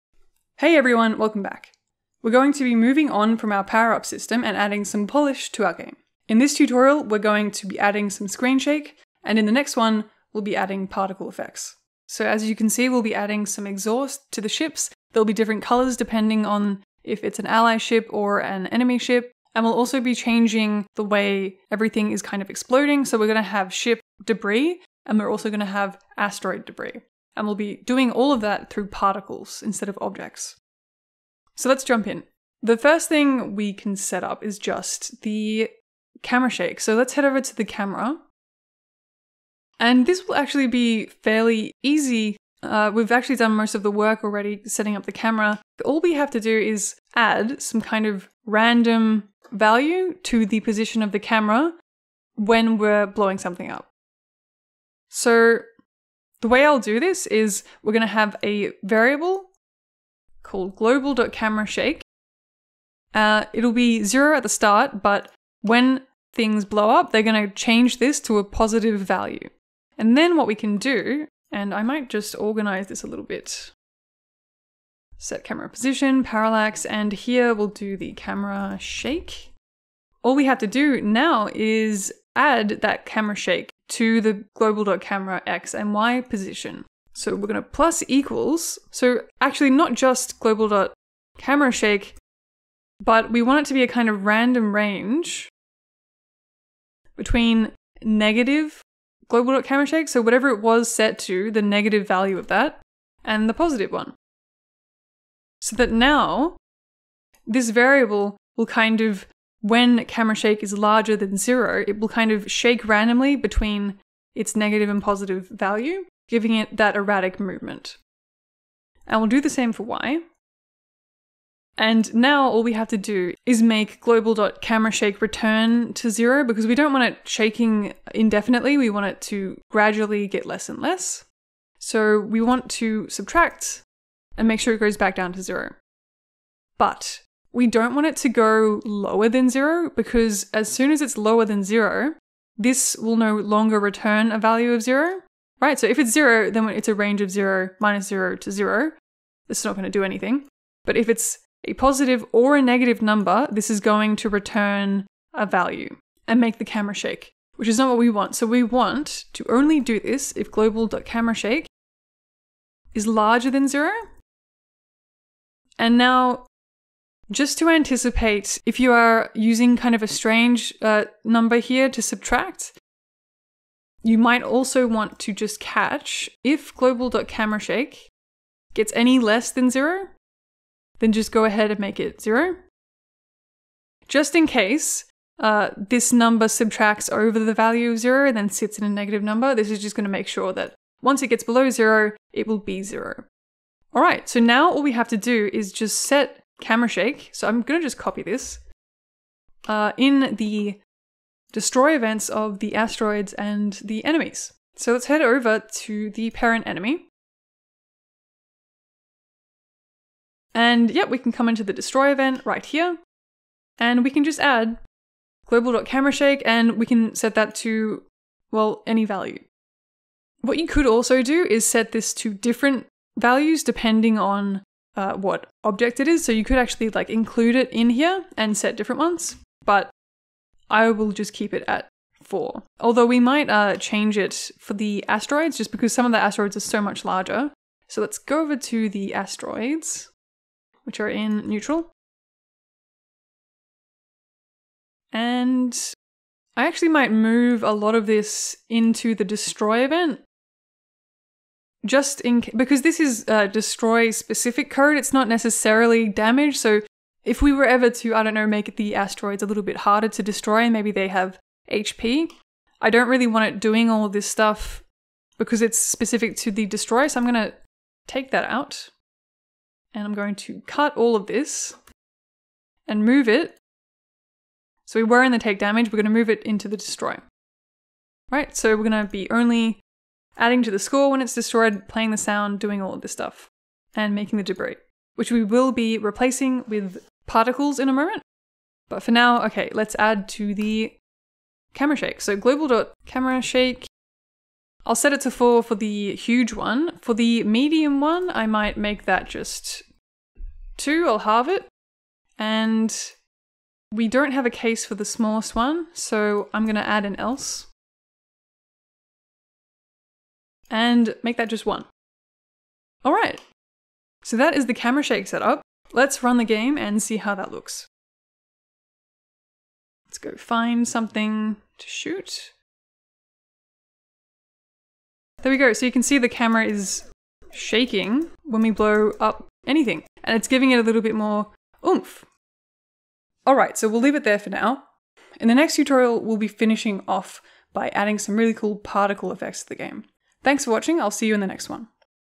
Space Mods - GML - Effects (1/2) - GameMaker Studio 2
FriendlyCosmonaut walks you through expanding your Space Rocks game with different advanced topics including Cameras, Power-ups and Effects using GameMaker Studio 2.

YoYo Games is based in Dundee, Scotland.

Follow @YoYoGames on Twitter and YoYo Games on Facebook.